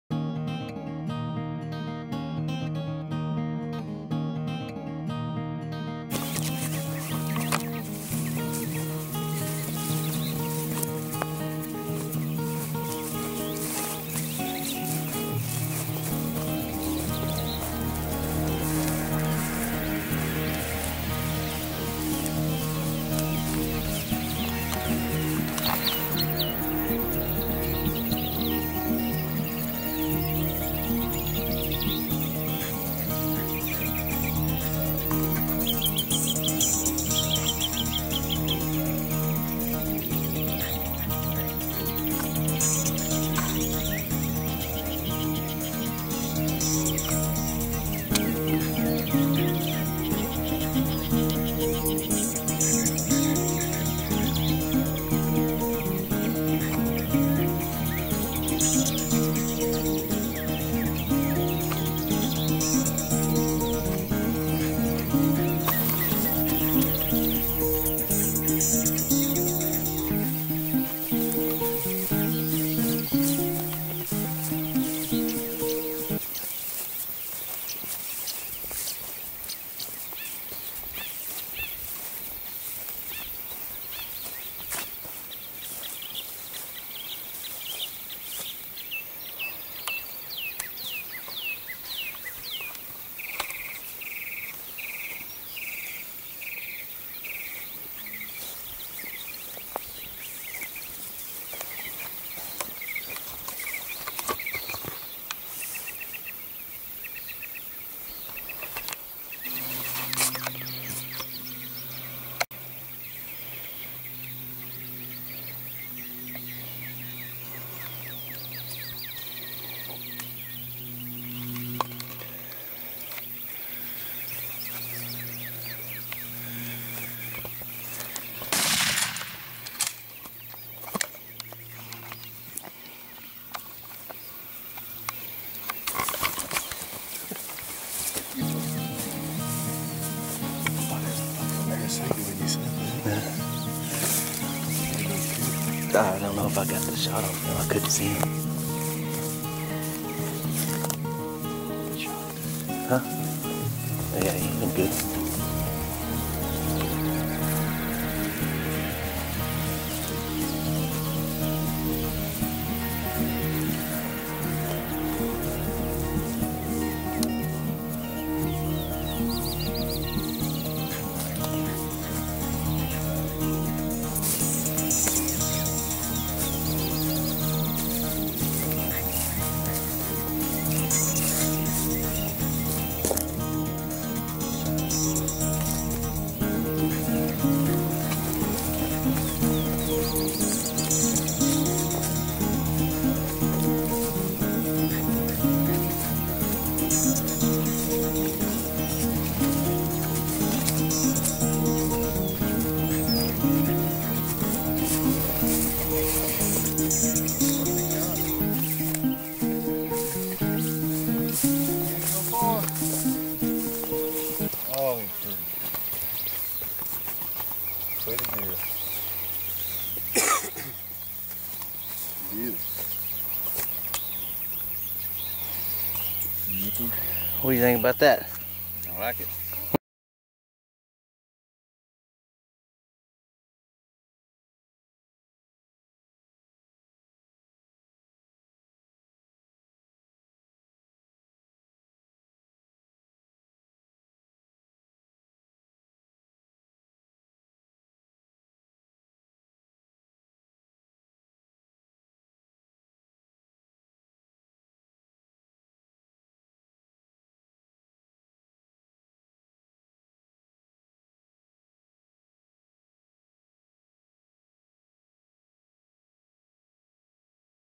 Spot and Stalk Axis Hunt
Hunting Axis during the Rut, not sitting in a Blind or over a feeder, but listening for them to Roar and slip in to get a shot.  After several failed attempts we able to get a shot on this great buck!  As we were heading to a Roaring buck it appeared this buck had the same thing on his mind, the wind was right and we spotted him seconds before he noticed us.  It was an awesome hunt!